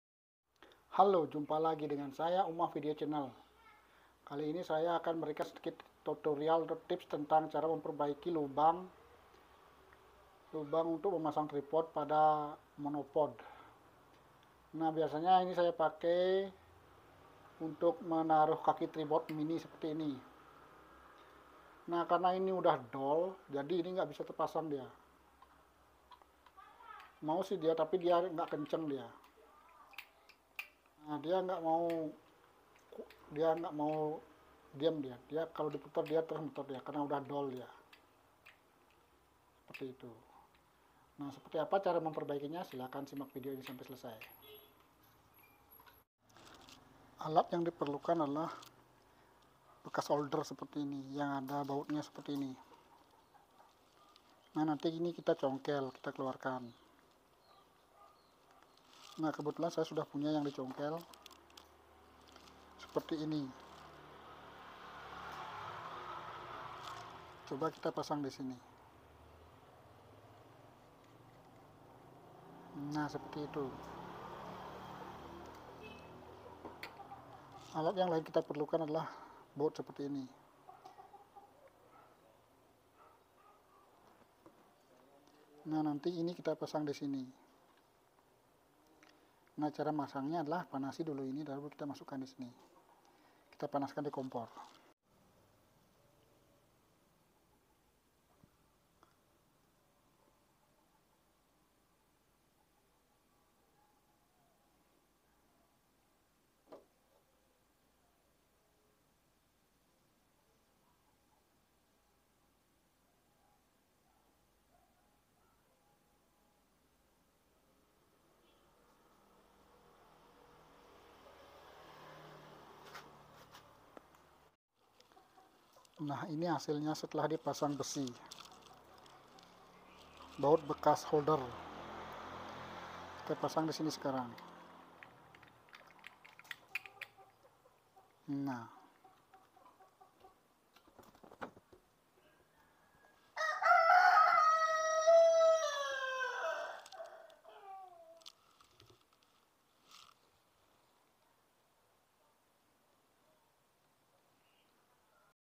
Lubang Mini Tripod pada Monopod Dol, ini cara perbaiki
pada bagian bawah monopod, biasanya ada lubang / drat untuk pasang Mini Tripod, nah kalau udah dol gak bisa dipakai, bagaimana cara memperbaikinya? silakan simak pada video ini.